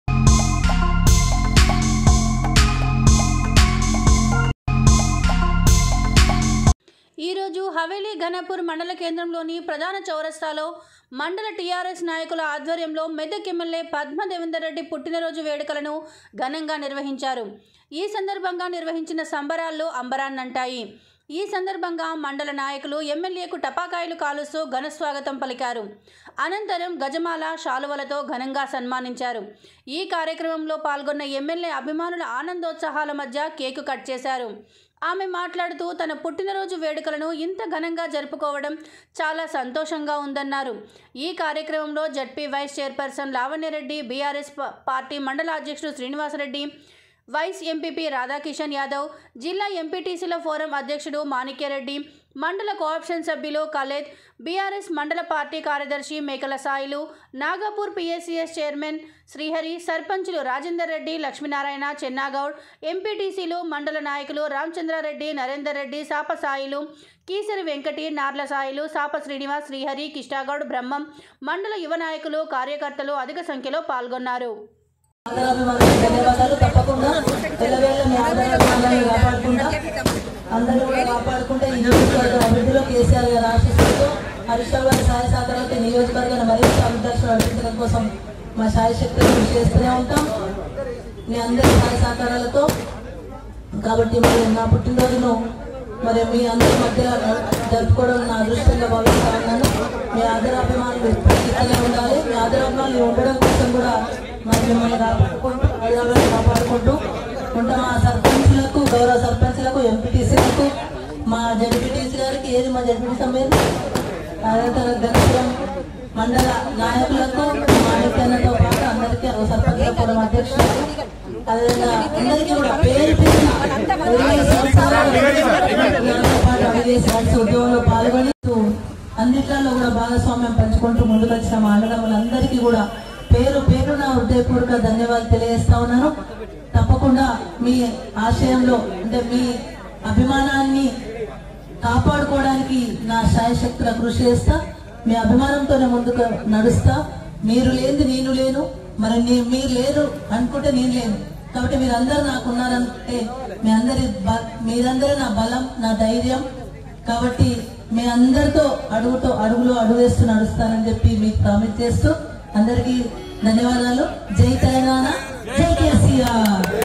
హవేలీ ఘనాపూర్ లోని మండల టిఆర్ఎస్ నాయకుల ఆధ్వర్యంలో ఎమ్మెల్యే పుట్టినరోజు వేడుకలు ఘనంగా నిర్వహించారు
ఈరోజు హవేలీ ఘనాపూర్ మండల కేంద్రంలోని ప్రధాన చౌరస్తాలో మండల టిఆర్ఎస్ నాయకుల ఆధ్వర్యంలో మెదక్ ఎమ్మెల్యే పద్మాదేవేందర్ రెడ్డి పుట్టినరోజు వేడుకలు ఘనంగా నిర్వహించారు